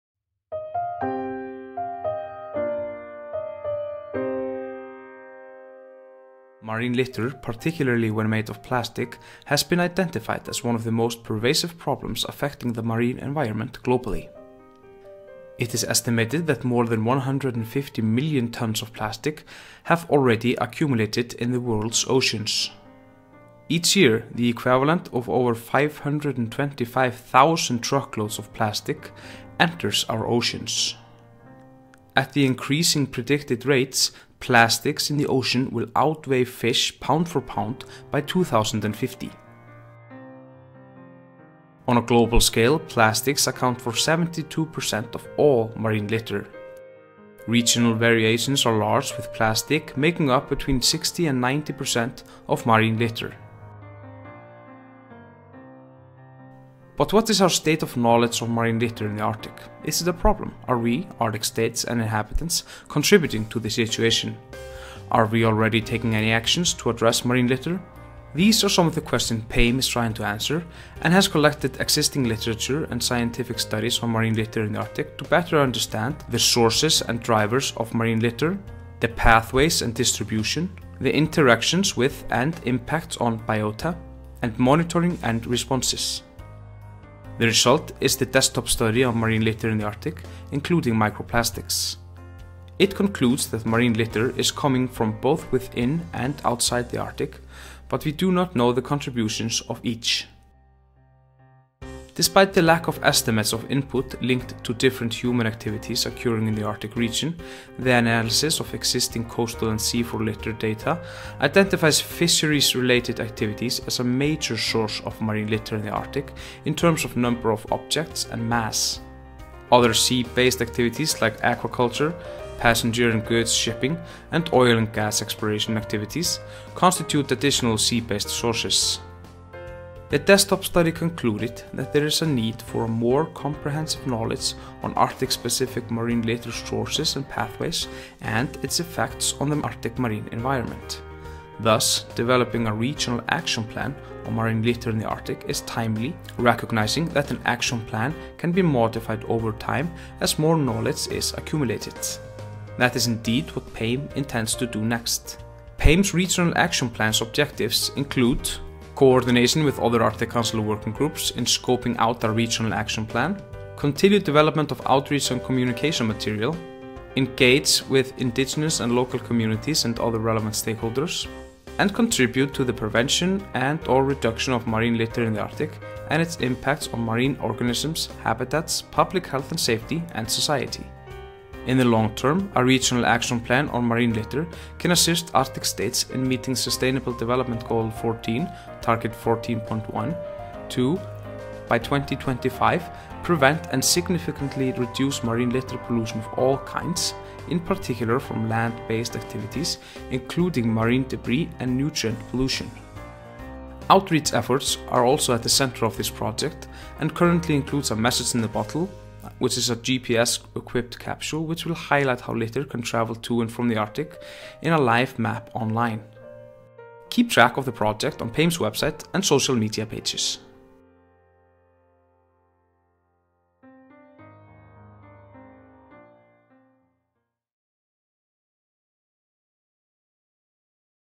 Marine Litter in the Arctic
A video about PAME's work on marine litter in the Arctic.